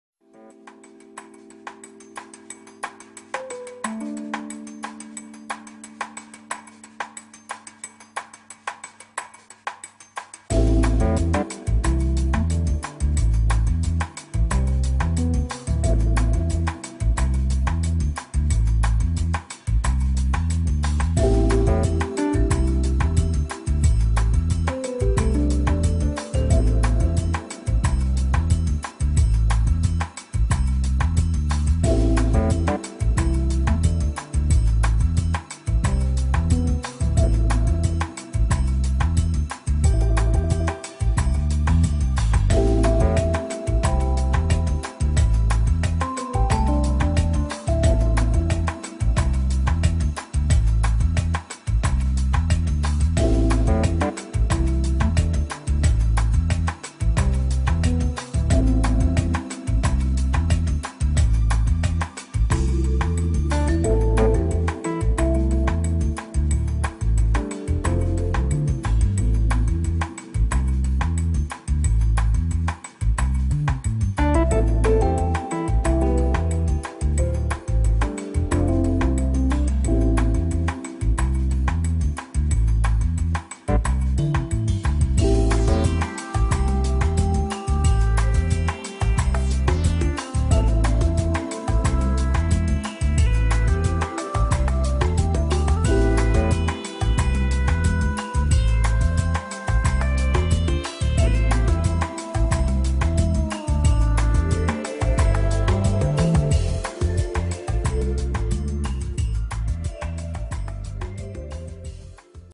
Hotel Riu Imperial Marhaba, Port el Kantaoui, Tunisie
Ce magnifique hôtel de style tunisien est situé en première ligne de la plage de Port El Kantaoui L'hôtel Riu Imperial Marhaba Port Kantaoui  vous propose un vaste choix de restaurants, de sports et d'animations. Une option idéale pour profiter du confort qui distingue les hôtels Riu.